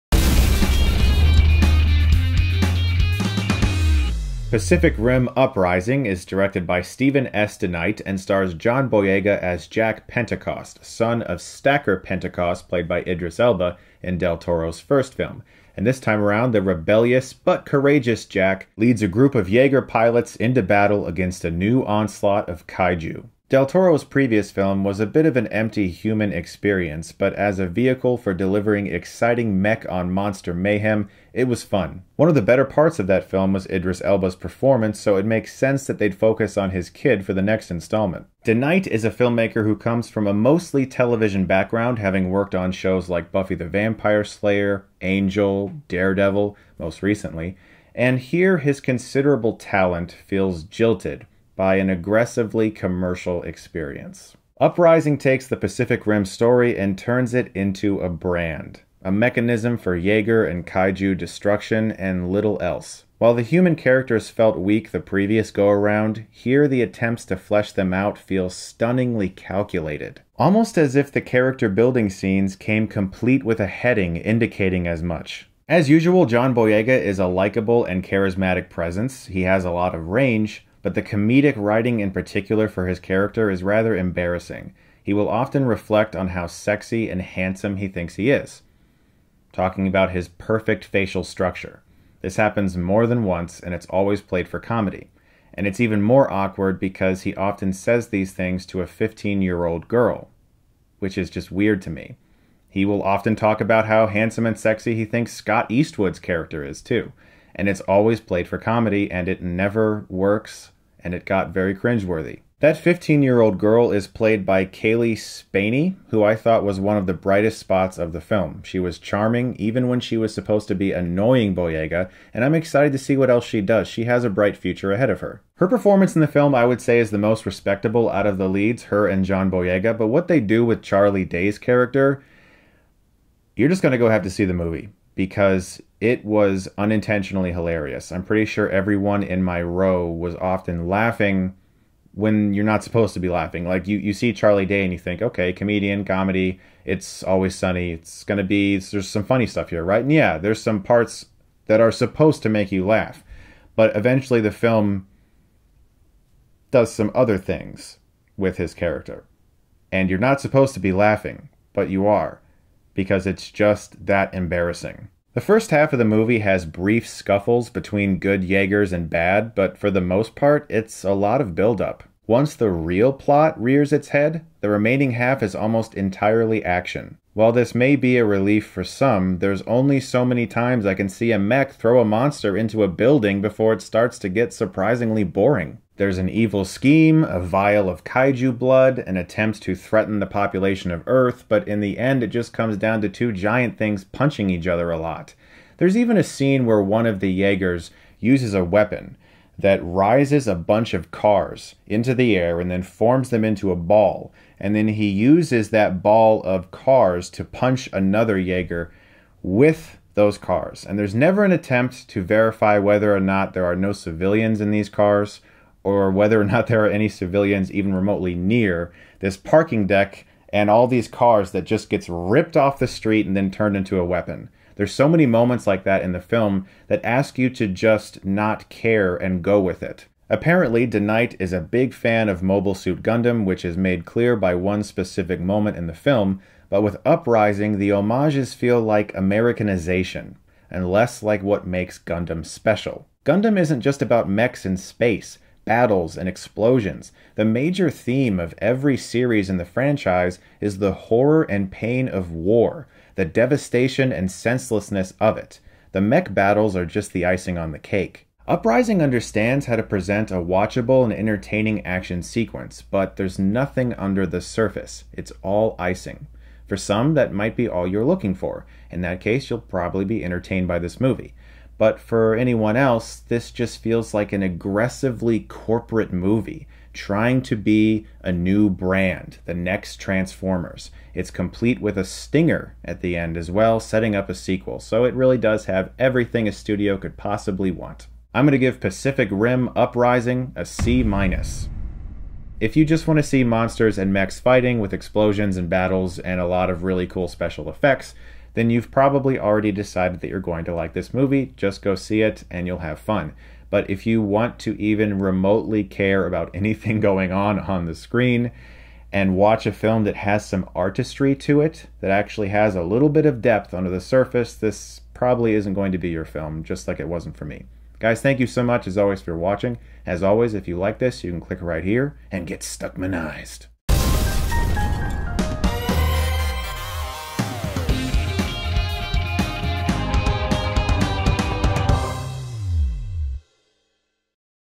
Pacific Rim: Uprising - Movie Review
Chris Stuckmann reviews Pacific Rim Uprising, starring John Boyega, Scott Eastwood, Jing Tian, Cailee Spaeny, Rinko Kikuchi, Burn Gorman, Adria Arjona, Charlie Day. Directed by Steven S. DeKnight.